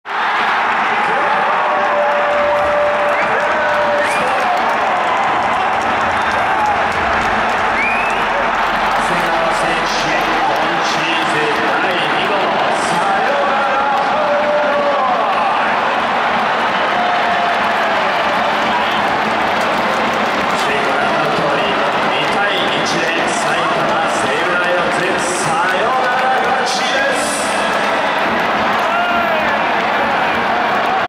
2023プロ野球 長谷川2号サヨナラホームラン(途中から)
ピッチャー 祖父江
打たれて意識が遠のくドラゴンズファンの心を表したかのようなピンポケ(カメラ故障)